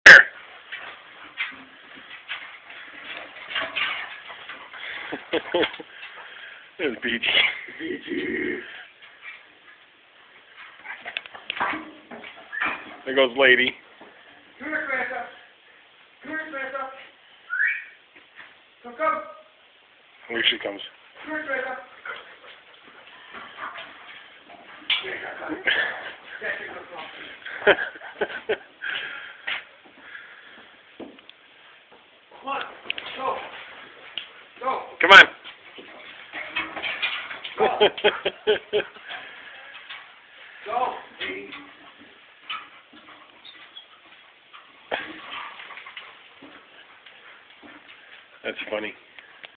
German Shorthairs running up stairs
Sammy and Lady running up the ladder!